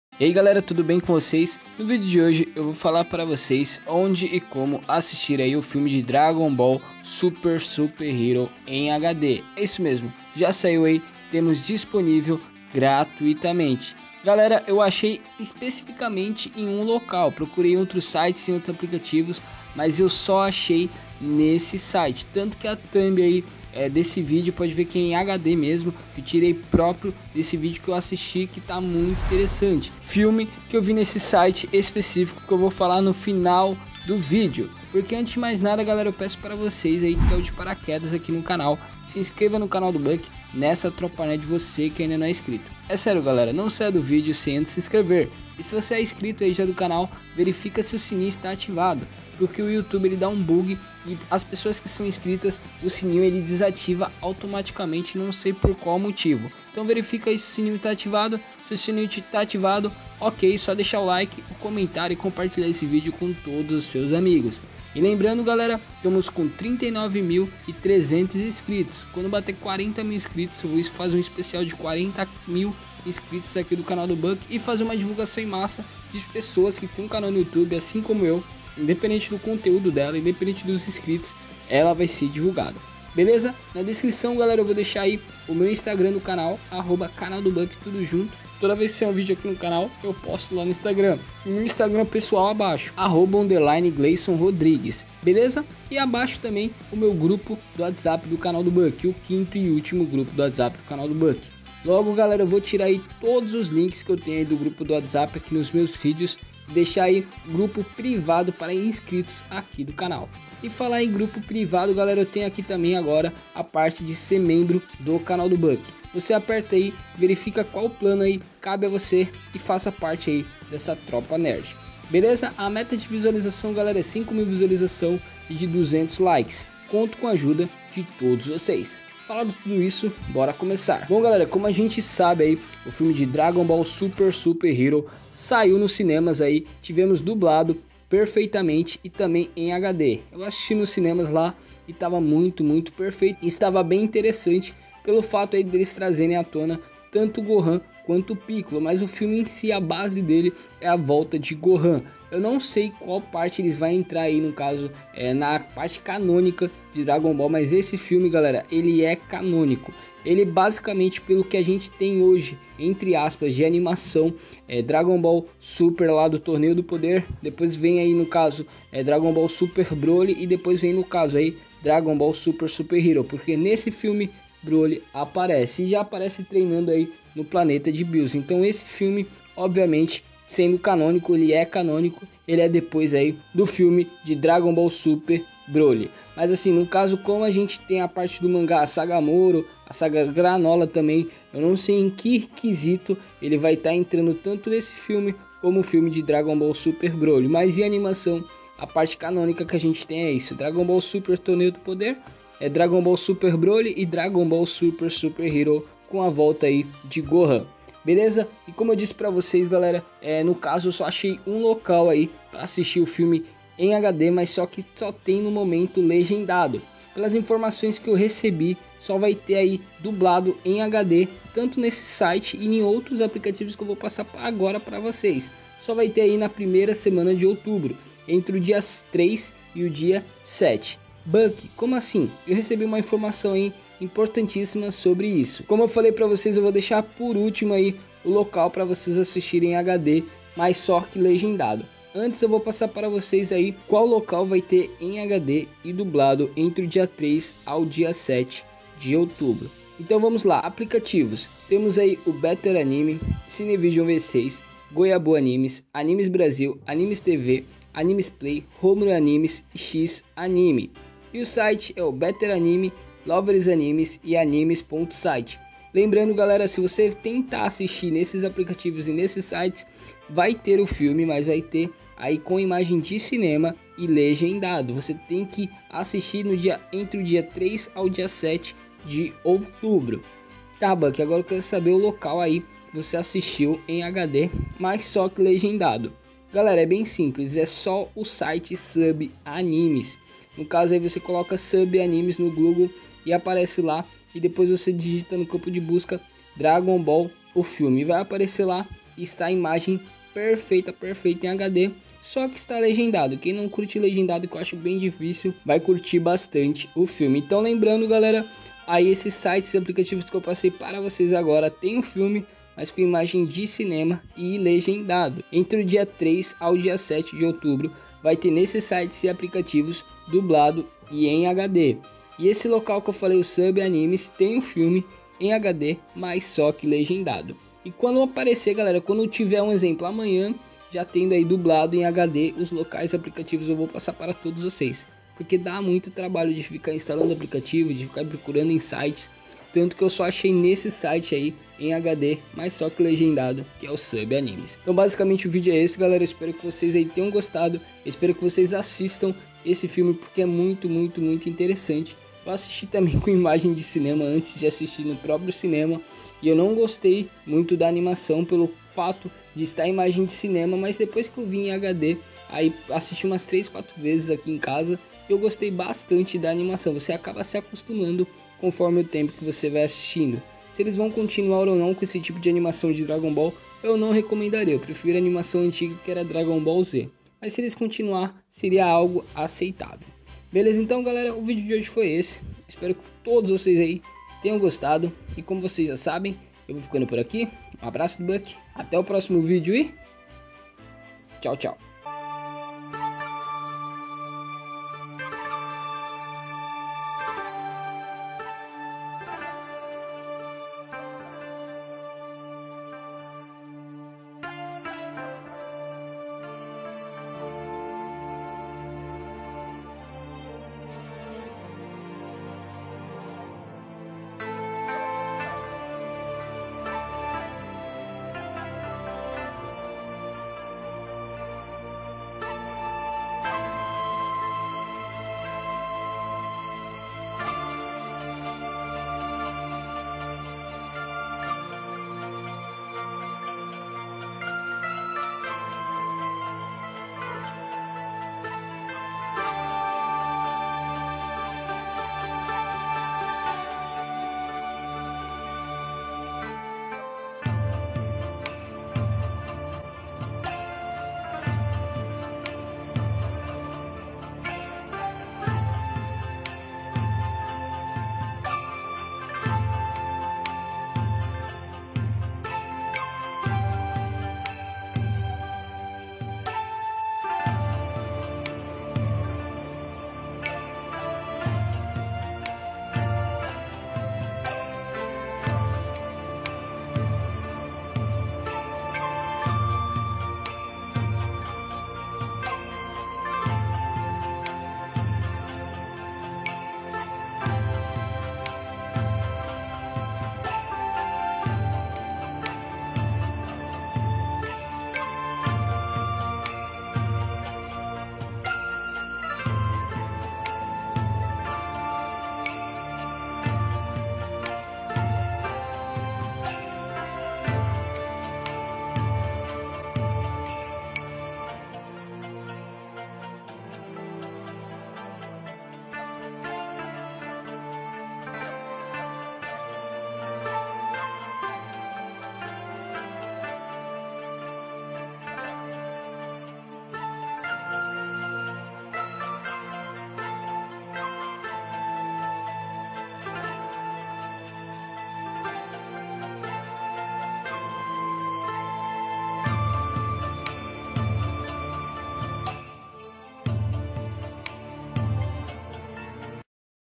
ONDE E COMO ASSISTIR AGORA DUBLADO EM HD - Dragon Ball Super Super Hero Filme 2022 DUBLADO FULL HD
Fala galera no vídeo de hoje eu vim falar para vocês onde e como assistir o filme de Dragon Ball Super Super Hero Dublado em HD, é isso mesmo, onde assistir Dragon Ball Super: Super Hero o filme! Vem com o Bucky❤️